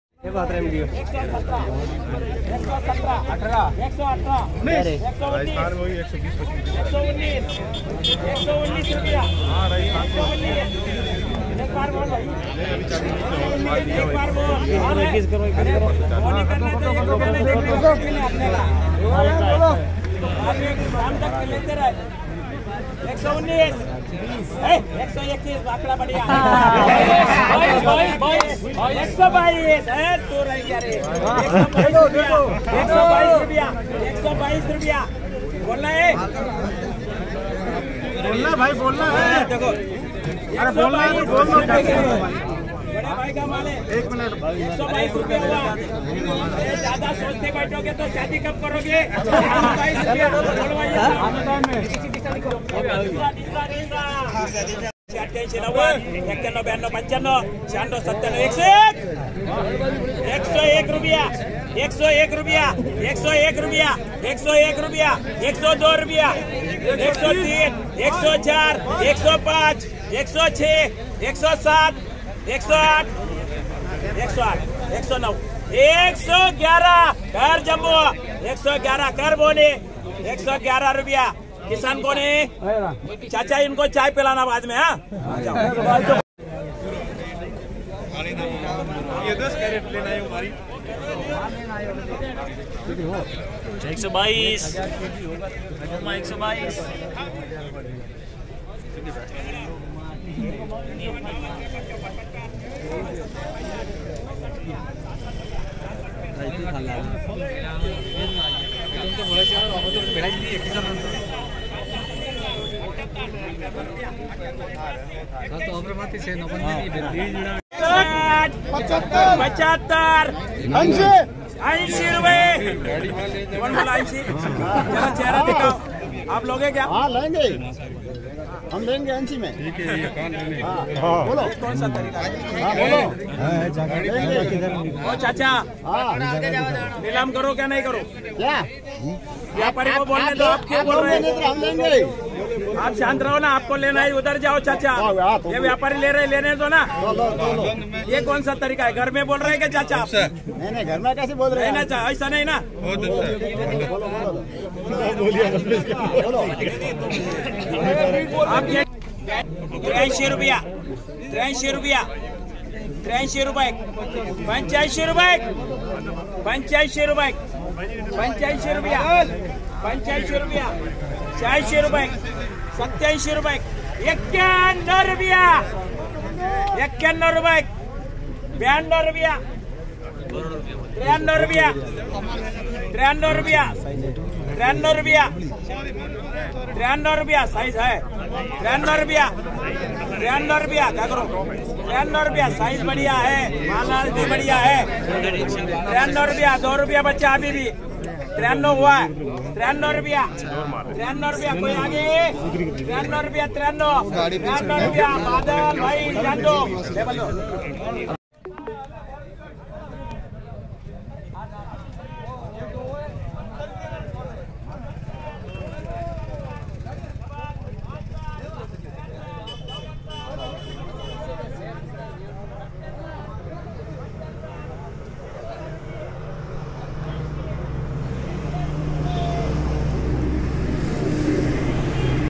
अनार मंडी जीवाणा का ताजा अपडेट,ऐसे लगती है अनार की बोली देखे वीडियो
जीवाणा में अनार मंडी के लाइव भाव,अनार की हराजी